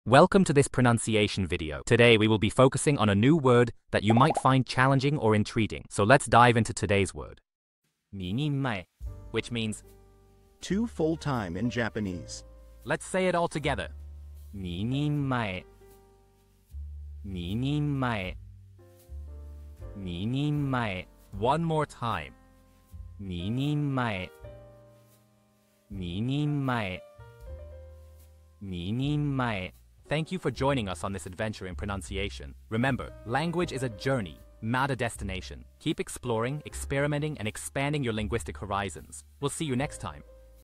How to pronounce 2hitomae | ２人前 (Two full -time in Japanese)
Master the Pronunciation of '2hitomae | ２人前' - which means : Two full -time in Japanese 📝 with @PronunciationJapanese 🎓🗣️ - [Your Guide to Japanese]

Learn to pronounce '2hitomae' perfectly in Japanese with our easy and engaging tutorial by @PronunciationJapanese! 📖📽️ In this comprehensive video, we provide you with native speaker examples 🗣️👂 and phonetic breakdowns 🎵 to ensure you get the pronunciation just right.